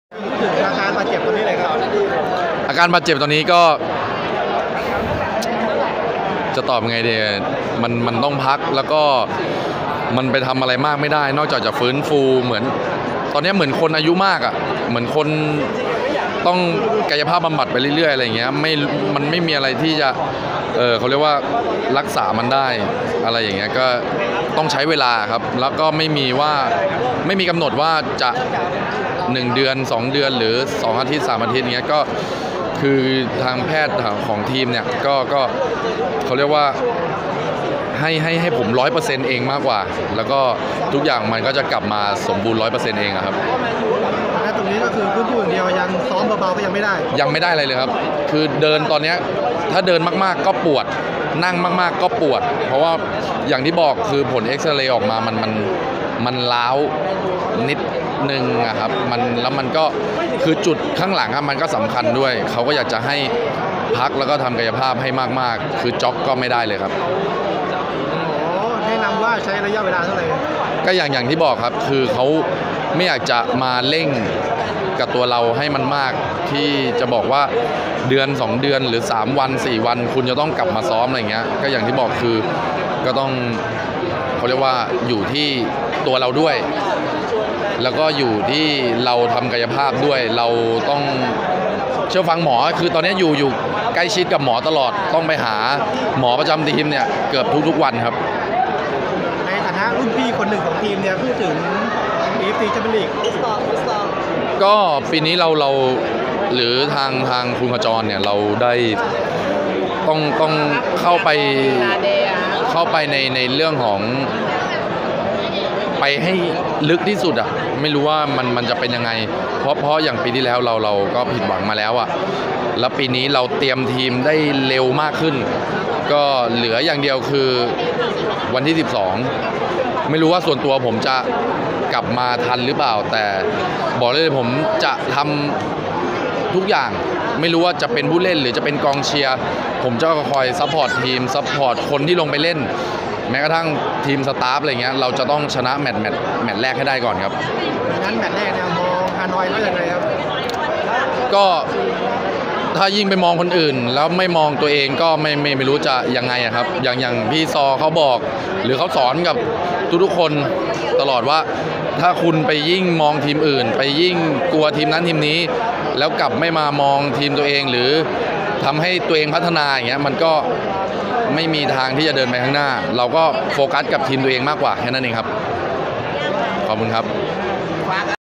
เห็นยิ้มๆ สรรวัชญ์ ยันเจ็บแทบน้ำตาไหล เผยถึง ACL เจอ Hanoi FC
สรรวัชญ์ เดชมิตร เผยถึงอาการบาดเจ็บที่ได้รับจากการรับใช้ทีมชาติไทย ในศึกเอเชี่ยนคัพ และโปรแกรมเอเอฟซี แชมเปี้ยนส์ ลีก ที่ต้องพบ ฮานอย เอฟซี จากเวียดนาม ในรอบเพลย์ออฟ